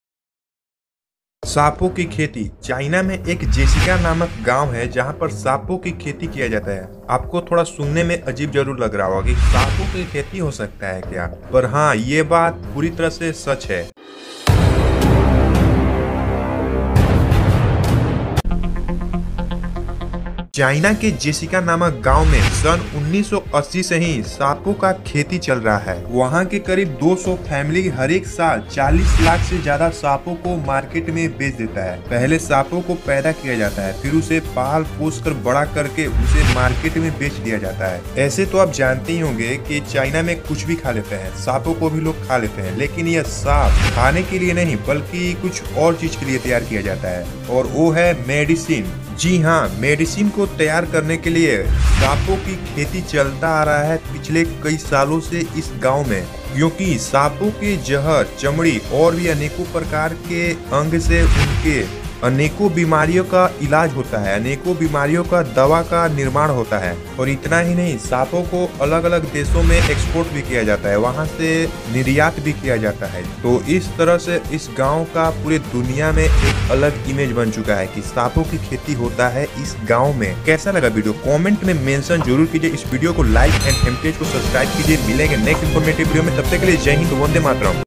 There Are Snakes Farming in This Village इस गांव में सांपों की खेती है - वजह जानते हो ? - FameTechz
In this video we discuss about There is snake farming in this village इस गांव में सांपों की खेती है Es gaw me sapo ki kheti ki jati hai

Keep Love and Support Friends...